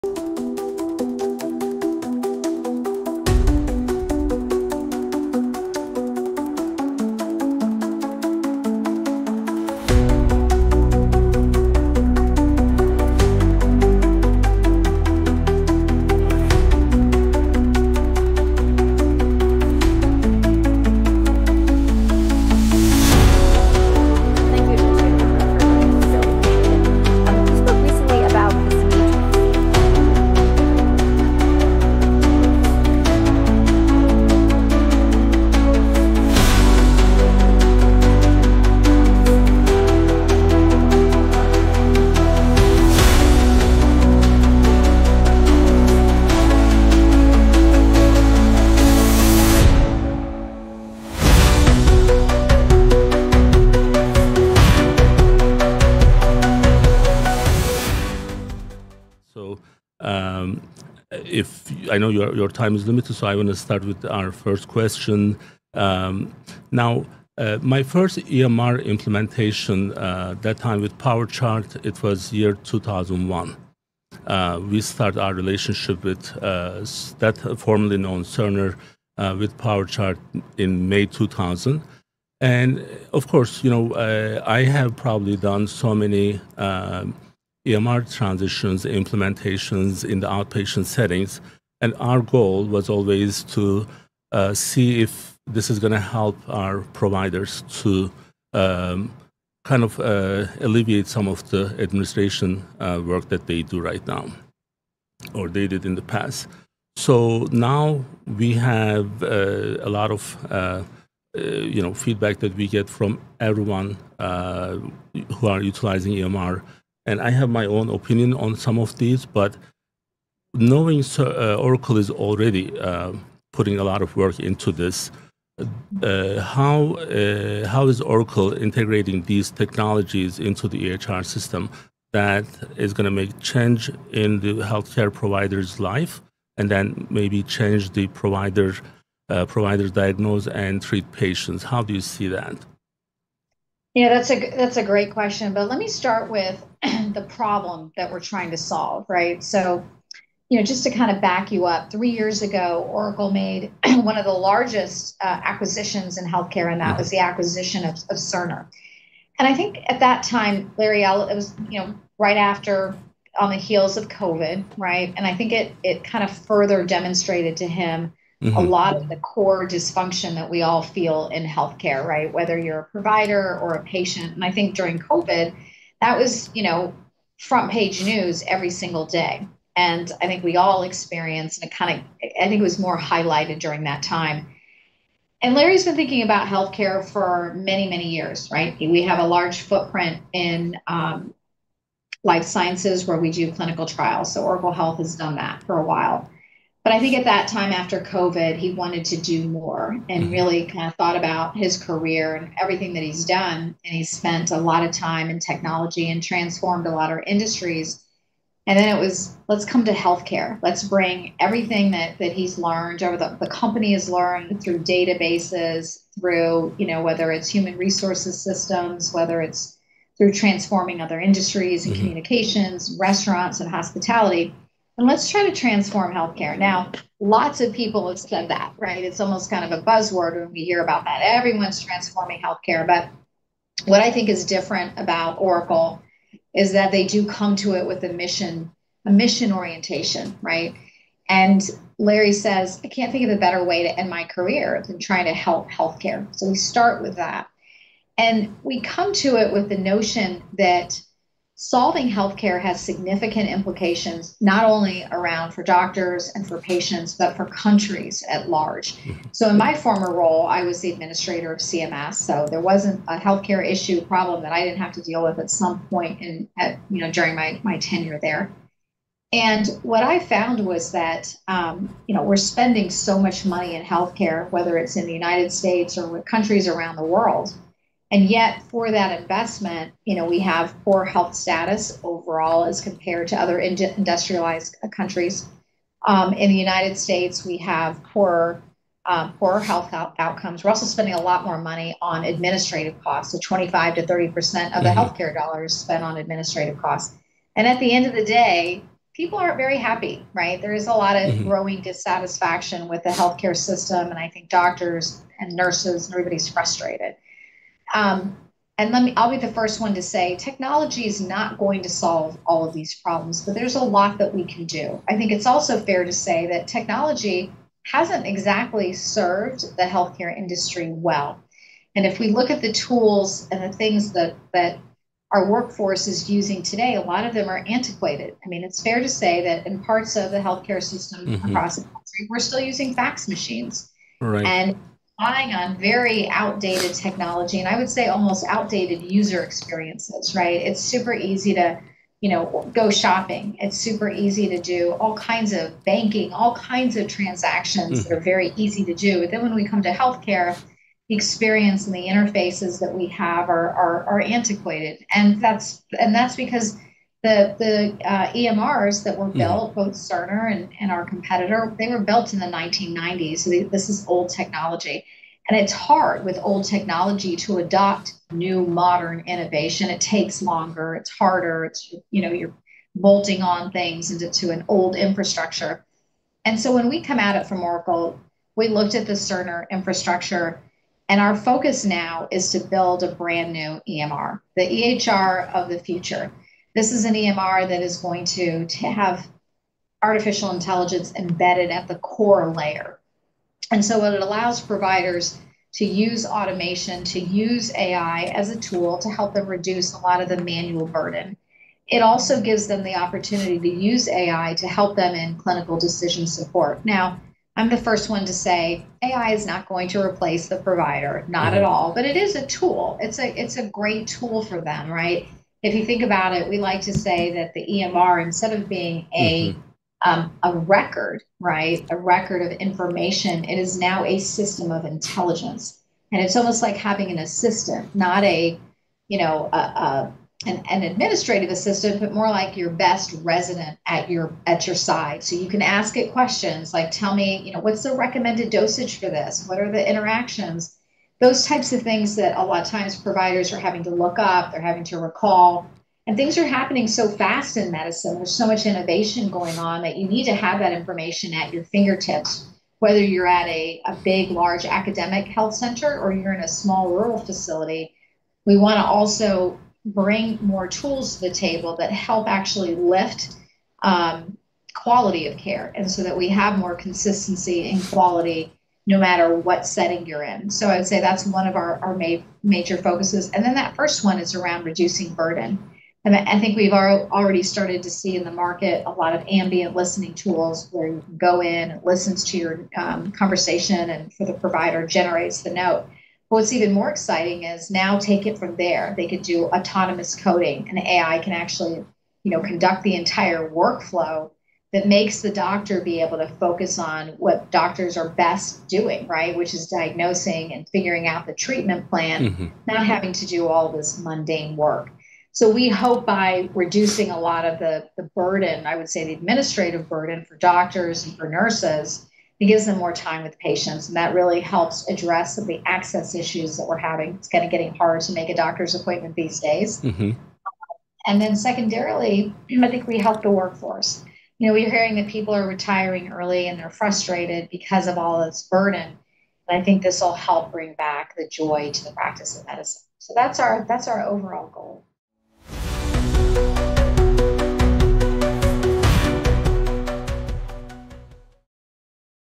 AI Meets Healthcare: Oracle’s Vision for a New EMR Era
UnitedTV UmAcoLive BariatricFriday In this forward-thinking episode of UnitedTV, Kemal Erkan sits down with Seema Verma, EVP of Oracle Health and former CMS Administrator, to discuss how Oracle is reimagining Electronic Medical Records (EMRs) for a new era — with AI at the core.

💬 Drop your thoughts and questions in the comments—we’d love to hear from you!

📍 Presented by UnitedTV and  @KemalErkan